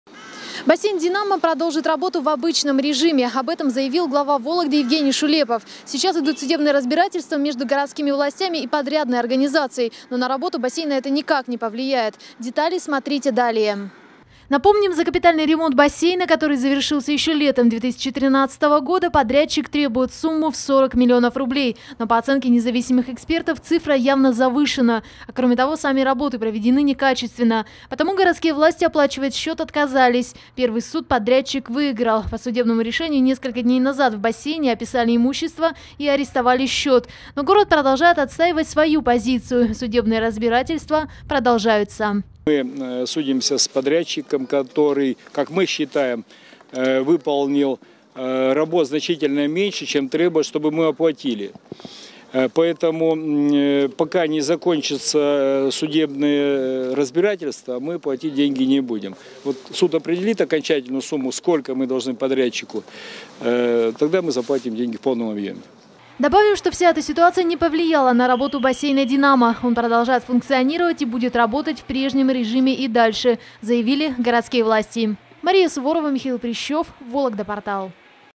Бассейн «Динамо» продолжит работу в обычном режиме
Об этом заявил Глава Вологды Евгений Шулепов. В настоящее время идут судебные разбирательства между городскими властями и подрядной организацией, но на работу бассейна это никак не повлияет.